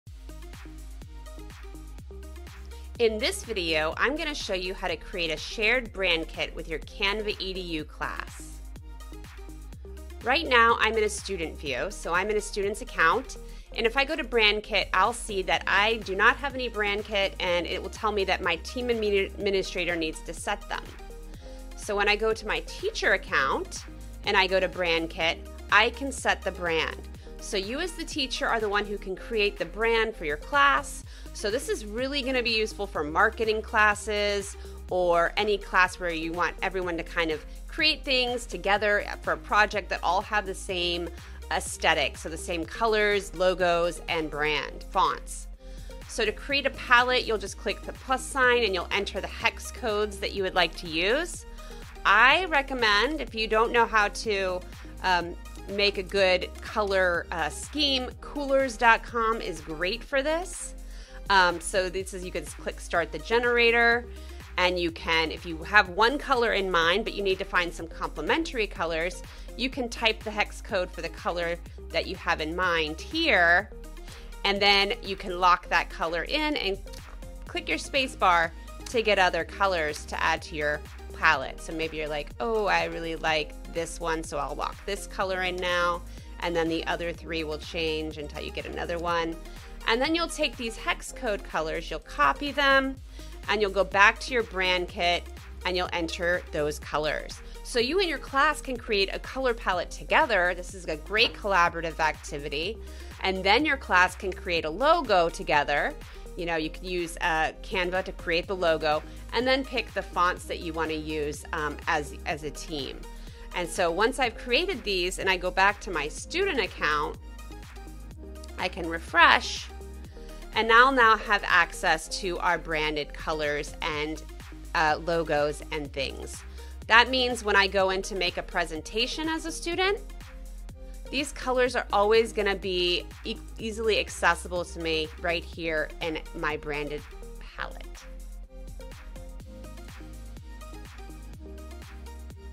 Canva Edu Brandkit
Presenter: Katie Fielding, Instructional Technology Coach
Certified Google Innovator, Trainer, and Coach
Canvas Certified Educator Level 1 & 2